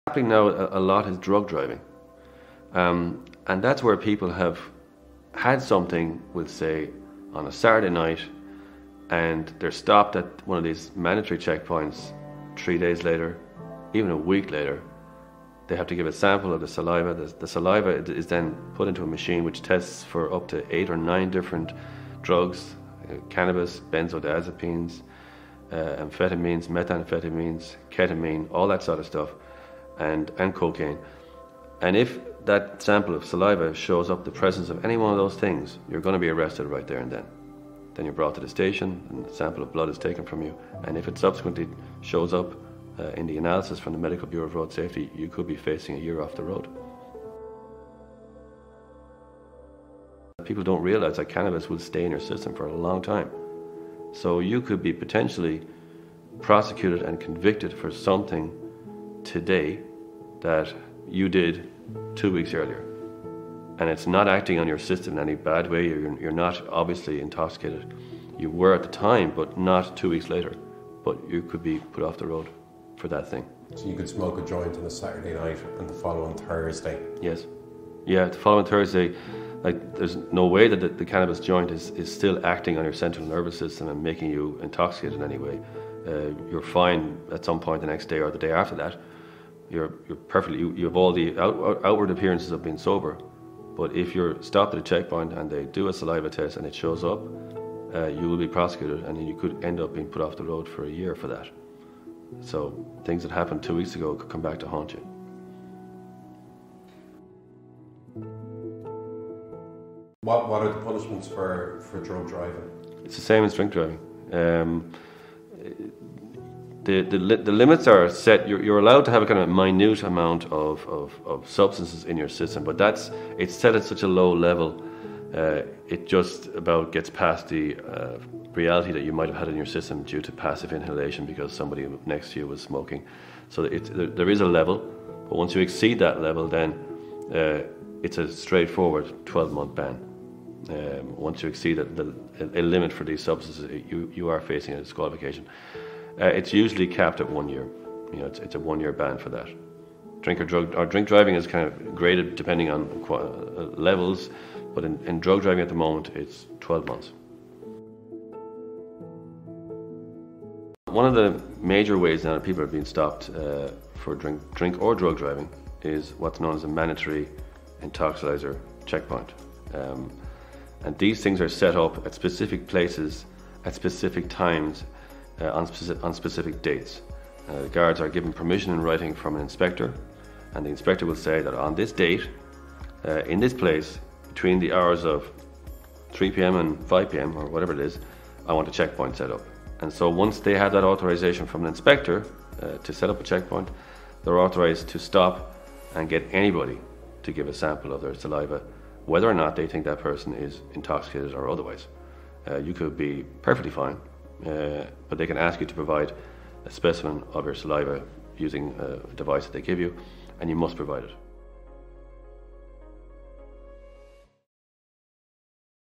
Should i get a solicitor for drug driving | What is the penalty for drug driving in Ireland? What...
stopped at one of these mandatory checkpoints 3 days later, even a week later. They have to give a sample of their saliva, the saliva is then put into a machine which tests